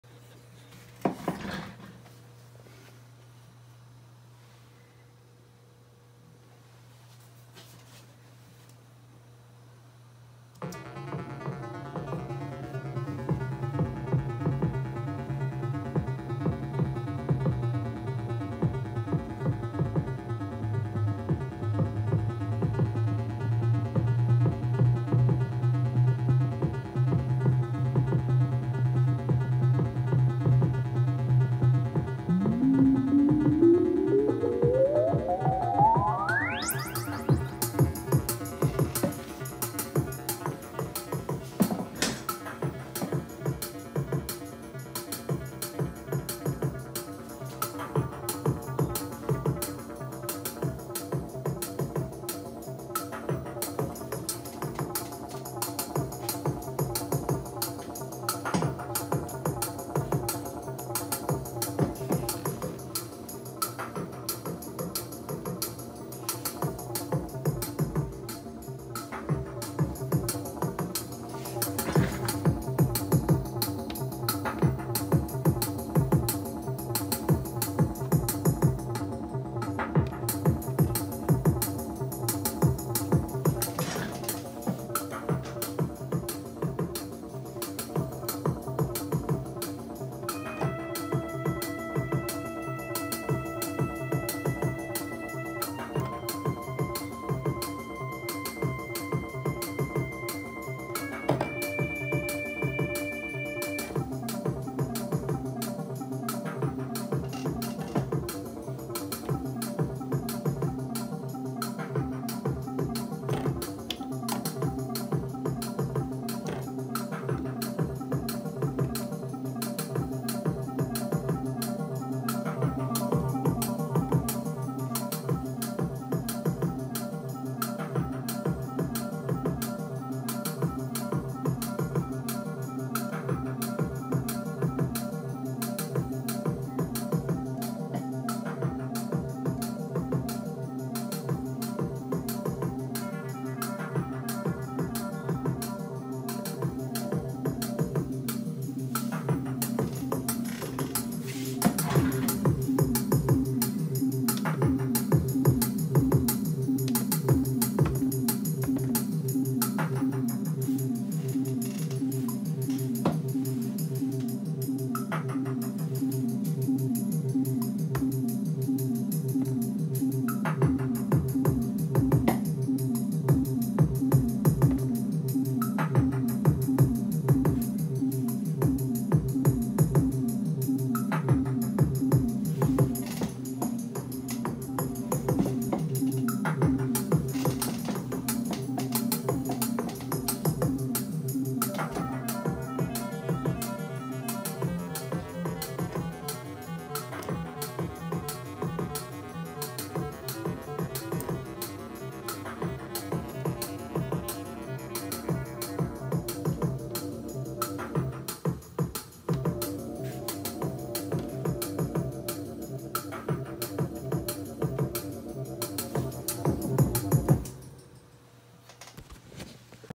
tempest matrix brute jam 1
just a quick jam while testing the set up. I used the iPhone, so the sound is a little wacky. I will be making a high quality multi camera vid soon.